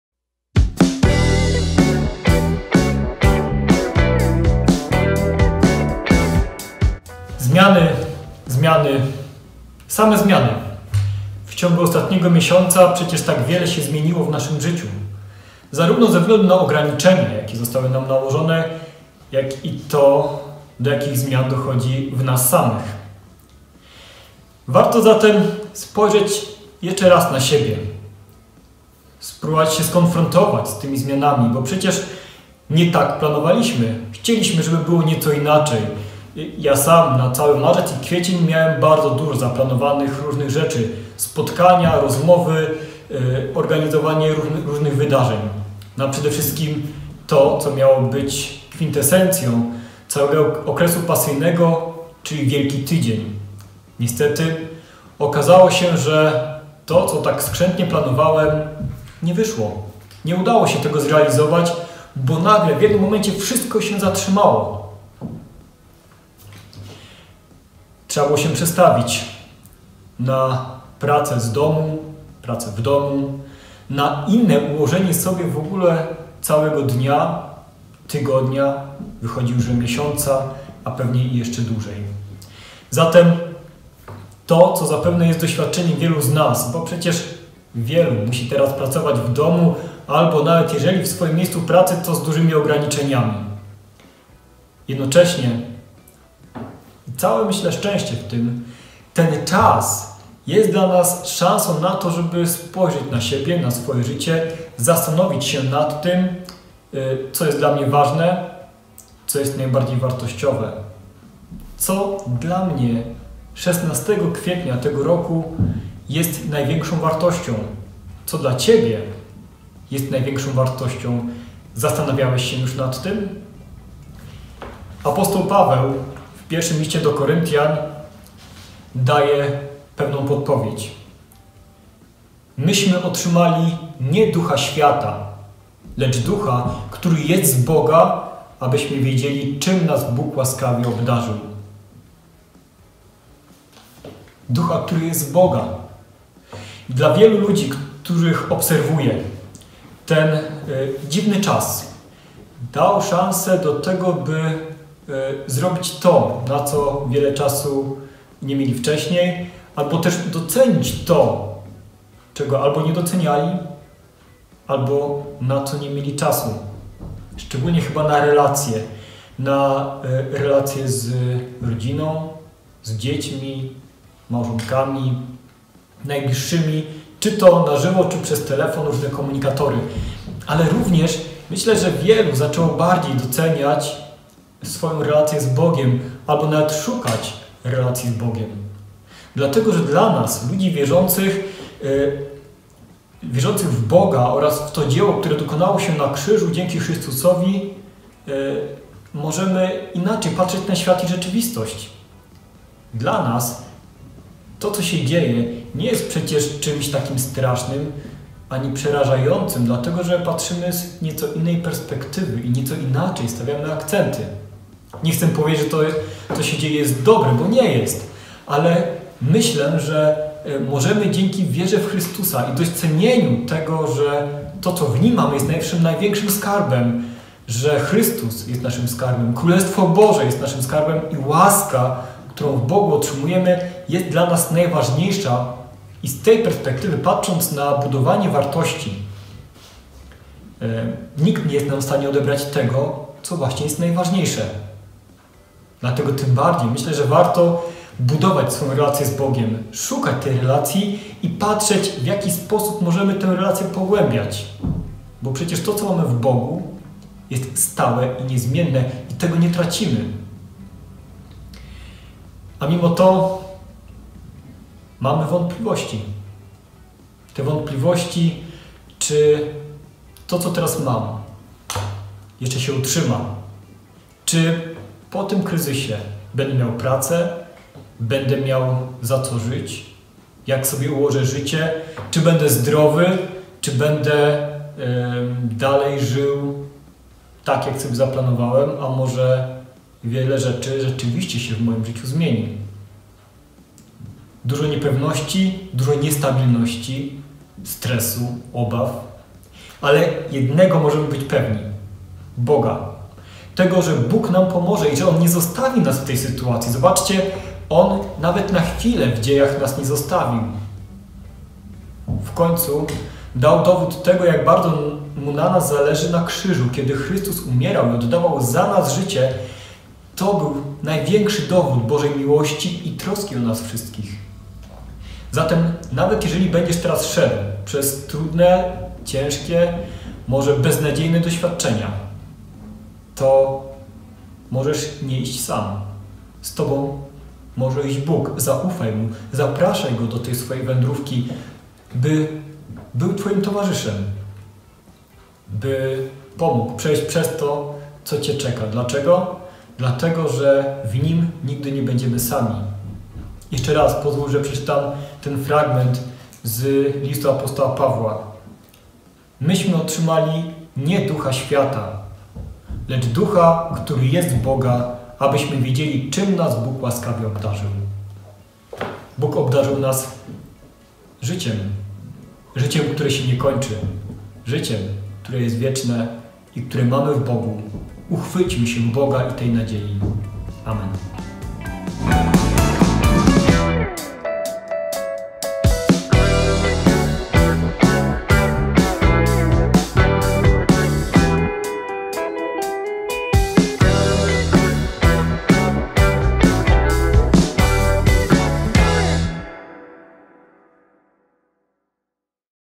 Codziennik biblijny, Słowo na dzień 16 kwietnia 2020 r.
Słowo na dzień 16 kwietnia 2020 r.
ks. Łukasz Stachelek, proboszcz parafii ewangelicko-augsburskiej w Olsztynie.